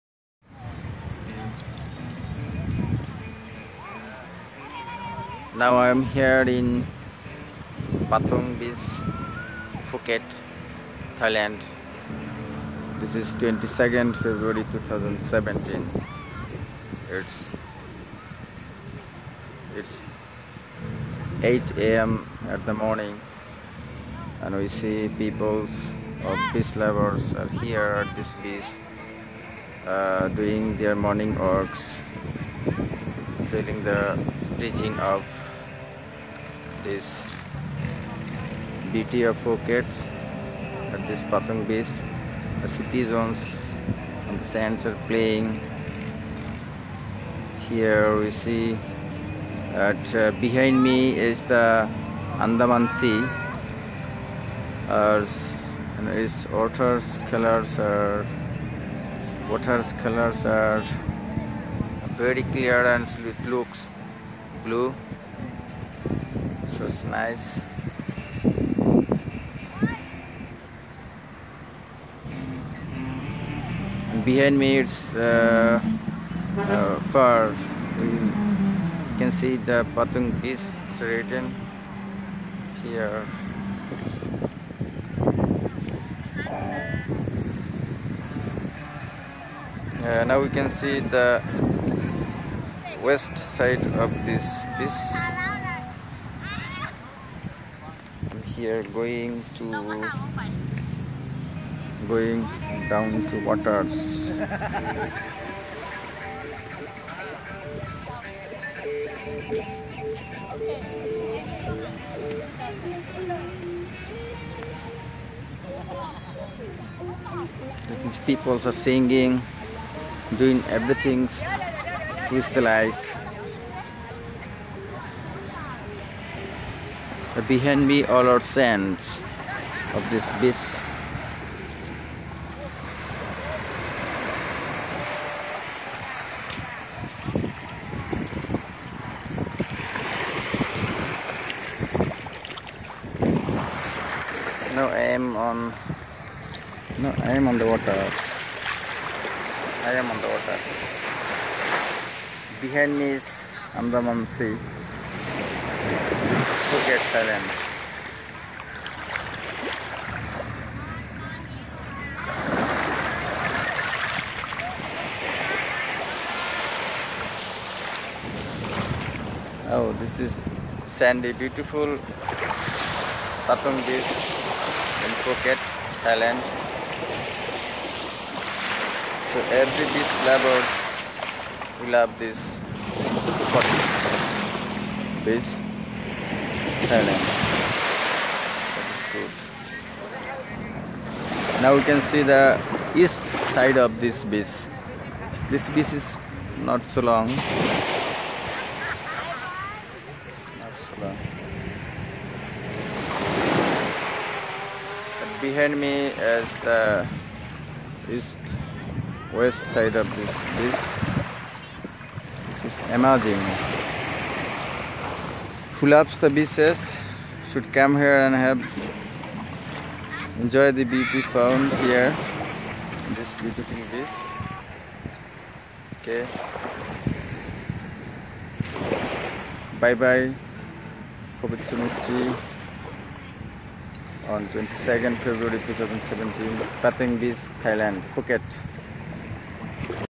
I am at famous Patong beach in Phuket
Pa Tong beach in Phuket, Thailand. 22 Feb 2017.